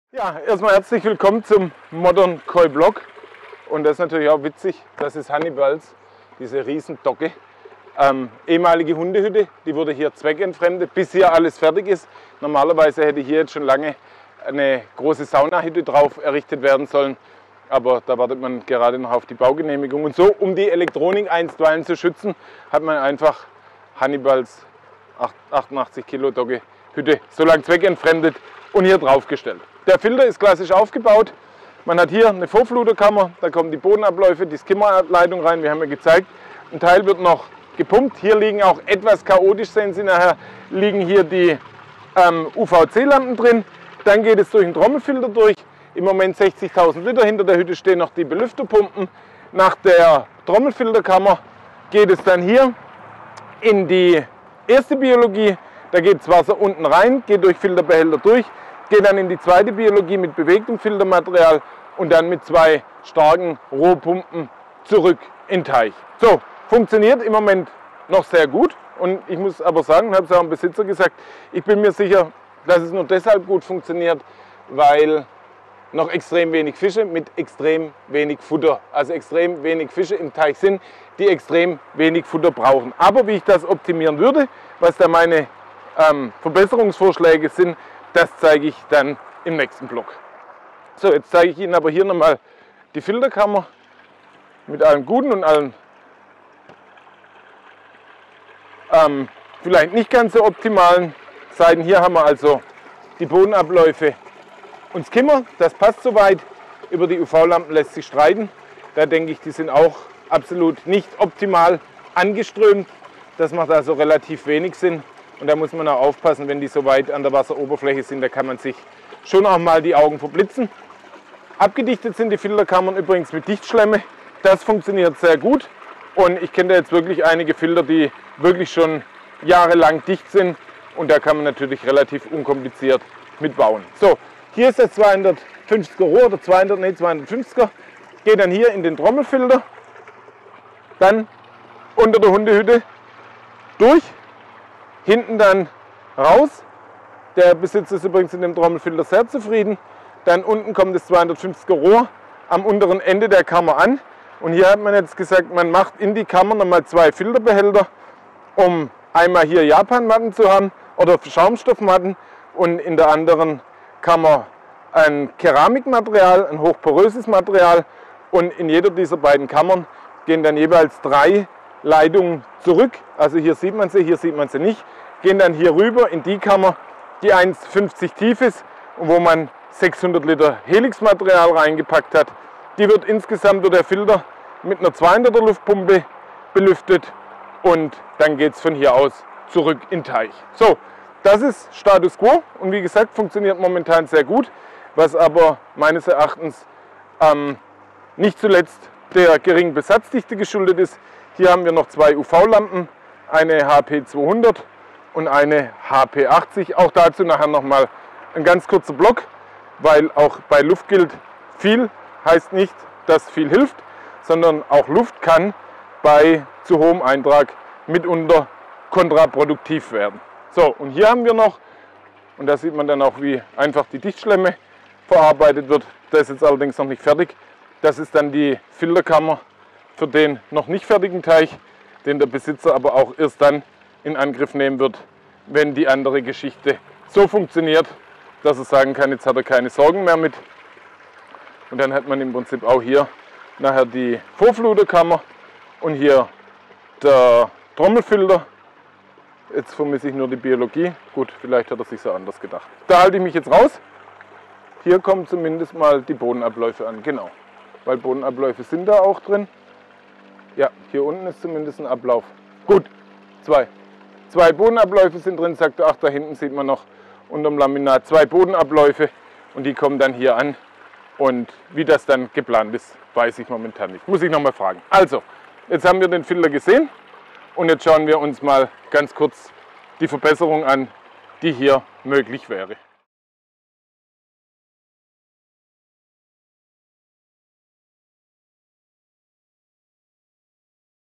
Modern Koi Blog #2051 - Ein selbstkonfektionierter Filter mit Trommelfilter
Auch an diesem Teich setzt man auf Trommelfilter mit anschließender Biologie die auf mehrere Kammern verteilt ist.